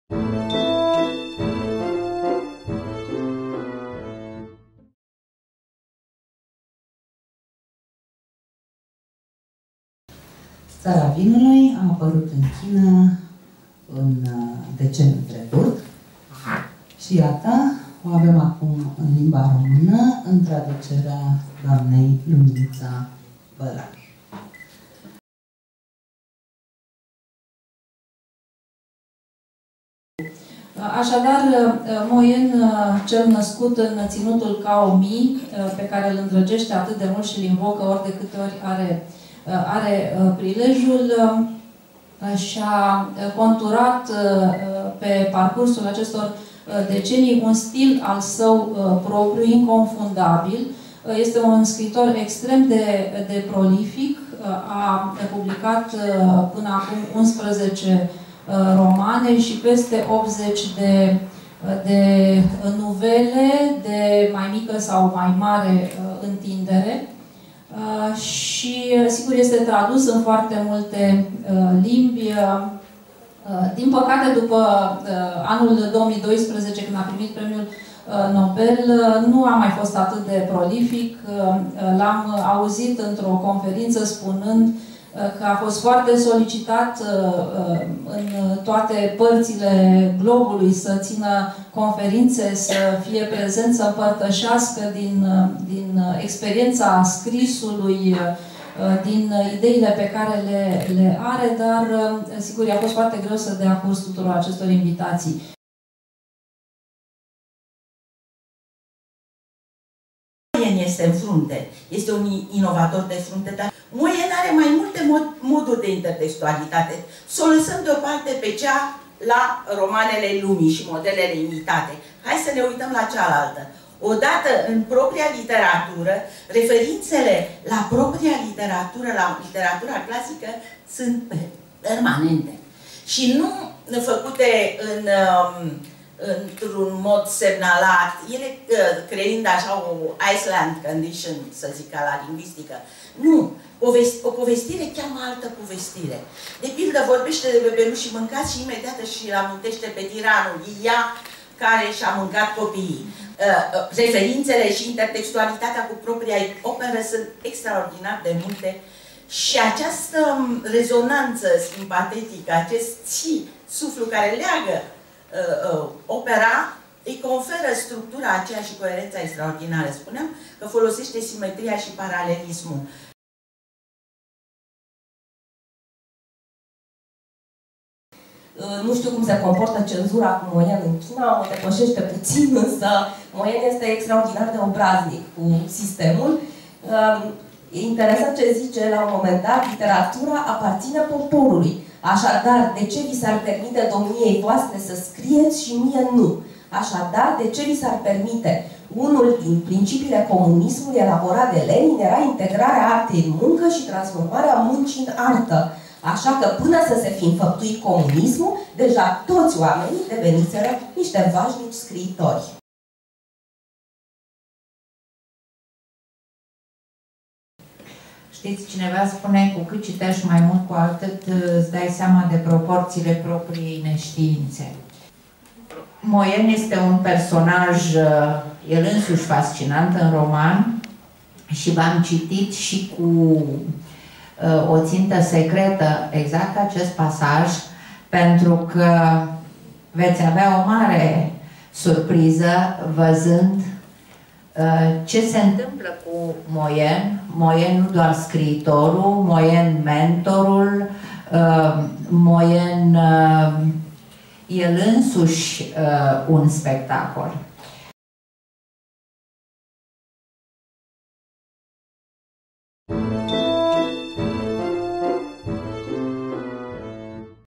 Seara Mo Yan - Humanitas
Țara vinului de Mo Yan este a cincea carte a scriitorului chinez care face parte din colecția Raftul Denisei, editată de Humanitas Fiction.